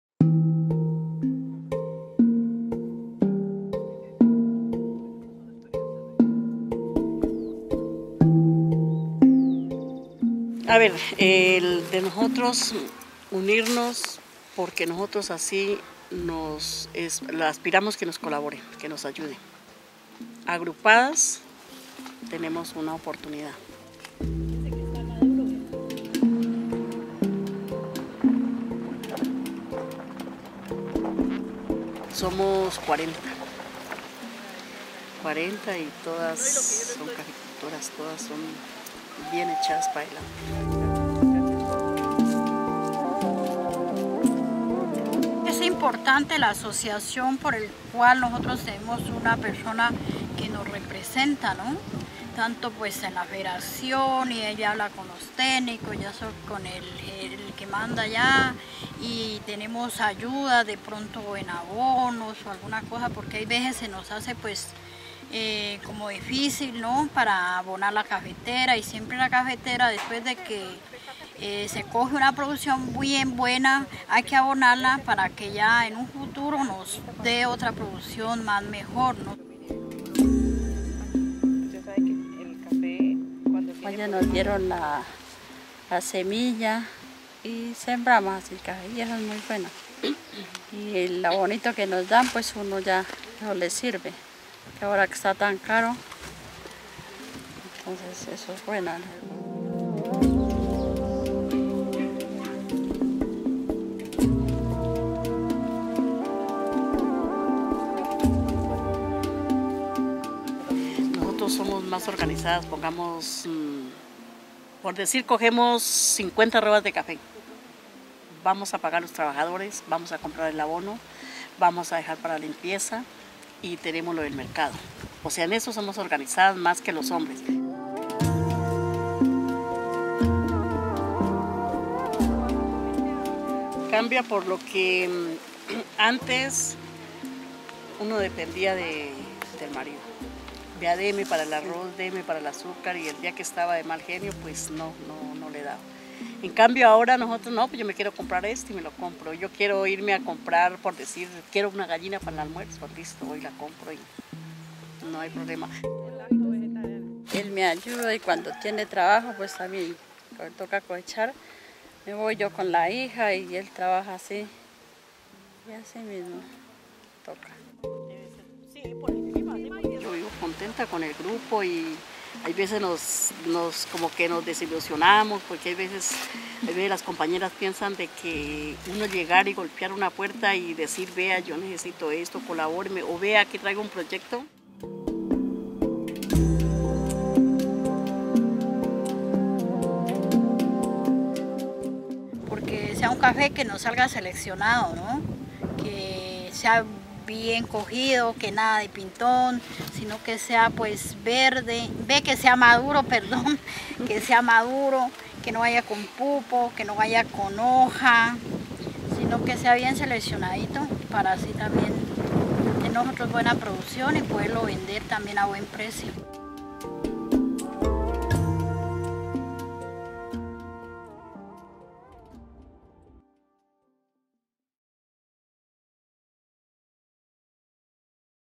Las Gaviotas, una organización comunitaria de mujeres cafeteras
Cuarenta mujeres del municipio de Timbío (Cauca) decidieron crear la asociación Las Gaviotas para, de manera colectiva, buscarle salidas a la producción del grano y mejorar sus condiciones de vida. Juntas han logrado sembrar 150 mil árboles de café y desarrollar procesos de buenas prácticas agropecuarias.